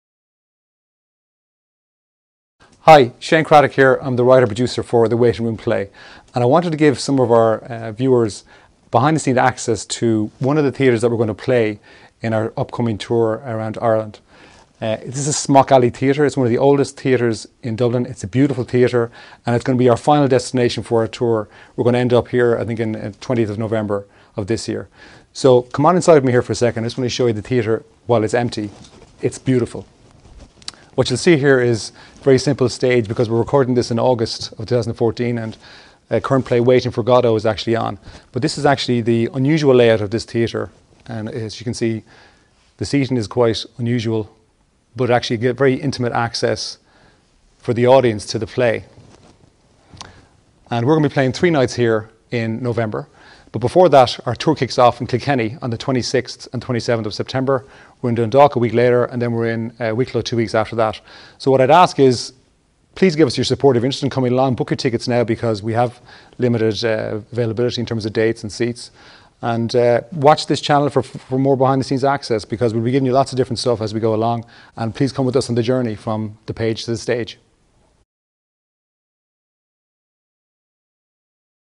Behind The Curtain with The Waiting Room - Ep 1
Writer/Producer of The Waiting Room, Shane Cradock, play gives some behind the scenes access to Smock Alley and talks a little about the forthcoming tour.